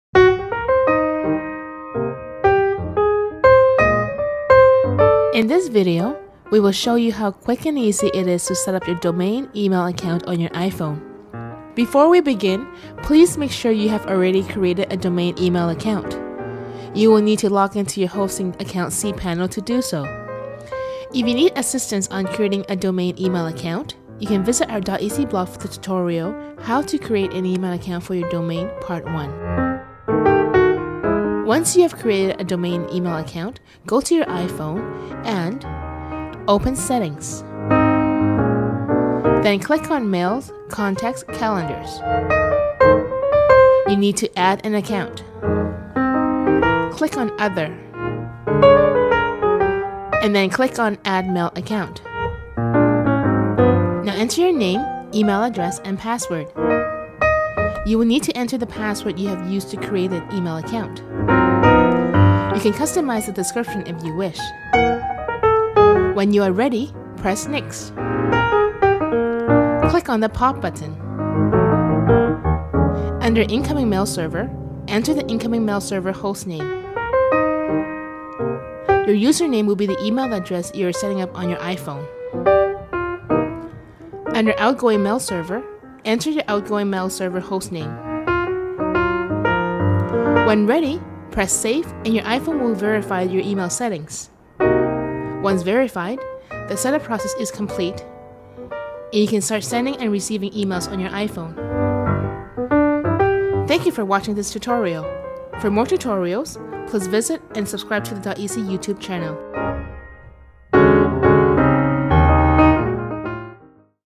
How to setup an email on iPhone
This video tutorial teaches you how to setup your email account for your domain on iPhone.